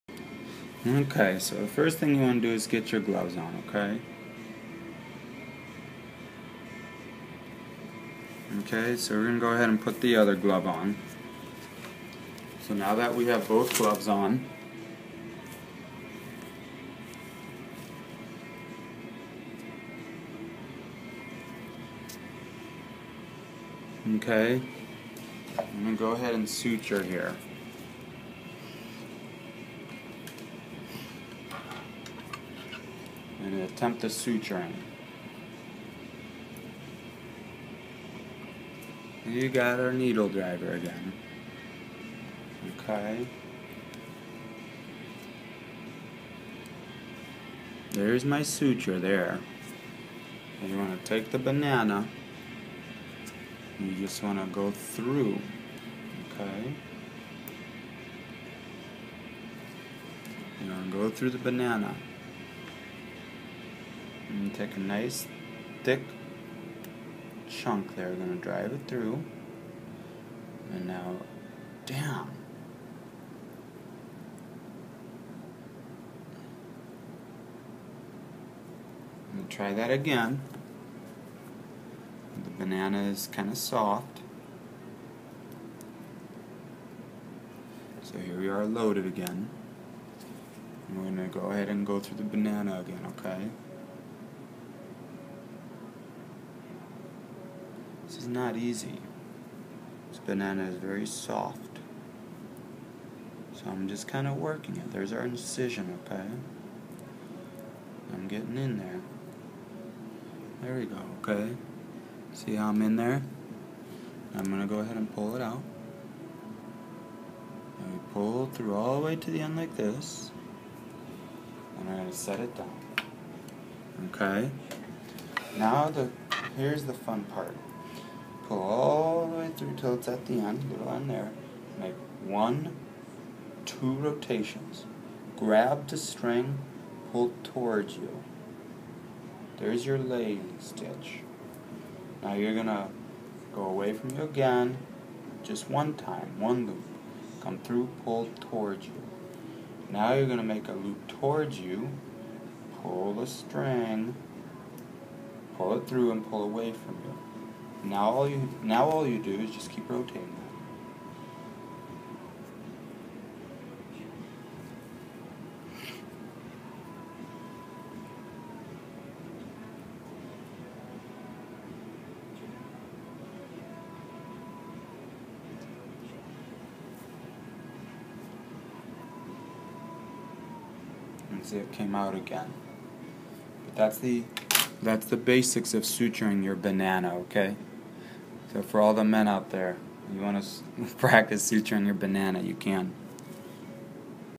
How to suture and tie square knot (on a banana)
A help video for 3rd year medical students on how to suture their bananas.